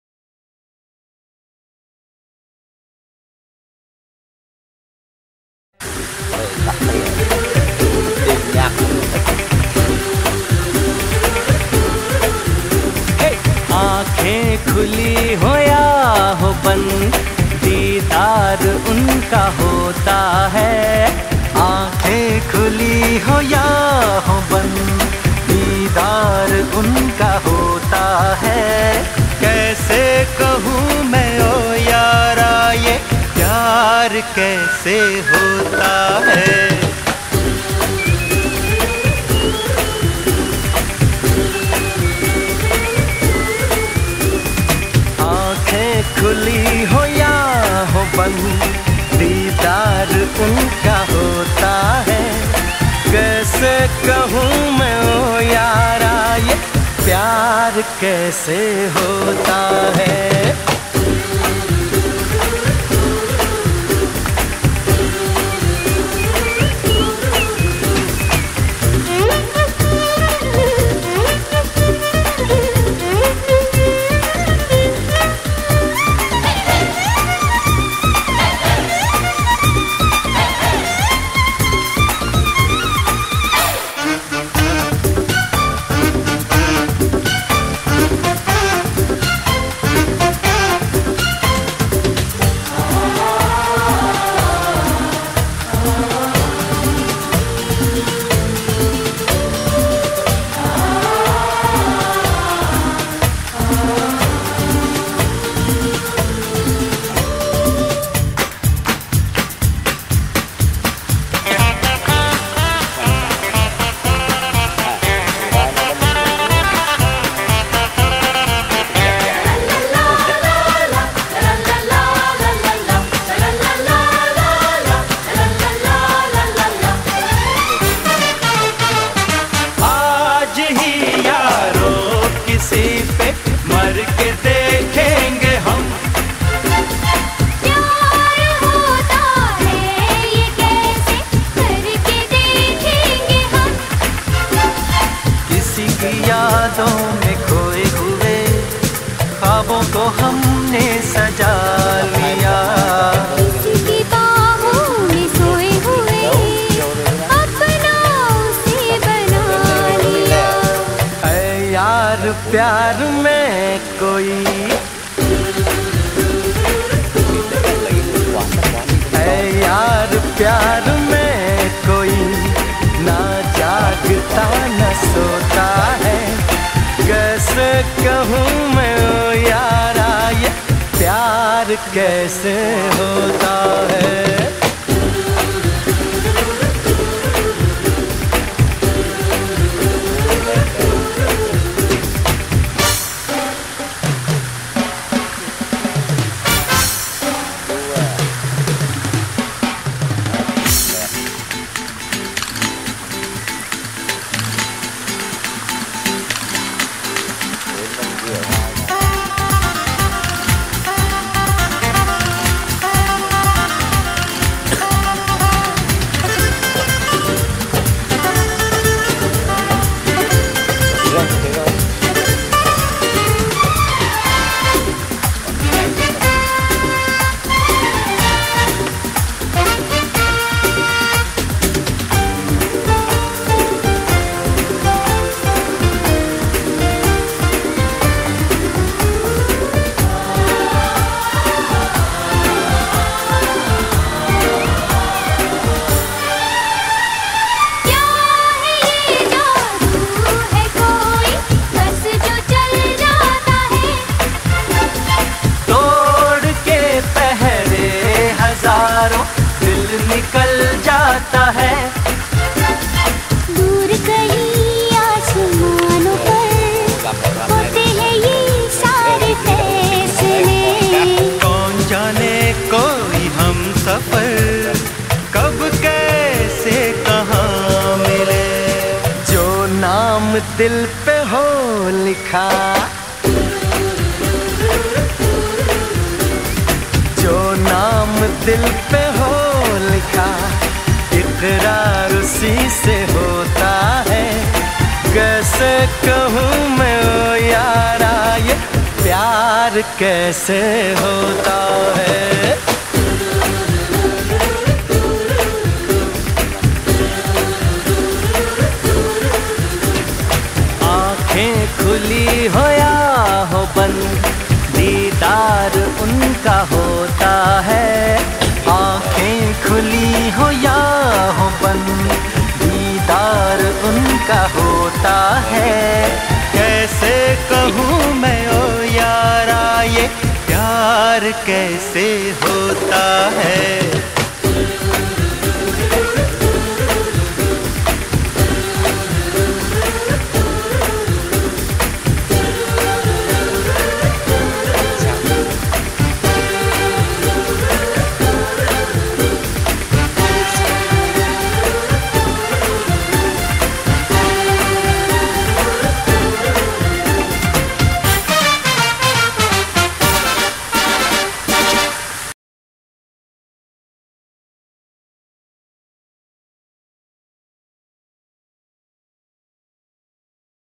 Đài phun nước trung tâm thương mại giải trí Cana (Bàu Cà Na), tỉnh Tây Ninh - Nhạc nước Ấn Độ
Hệ thống đài phun nước nghệ thuật, nhạc nước tại Trung tâm Cana. Xem chi tiết tại
Công ty Hải Đăng chuyên thiết kế, thi công, lắp đặt hệ thống đài phun nước, phun nước nghệ thuật theo nhạc - nhạc nước.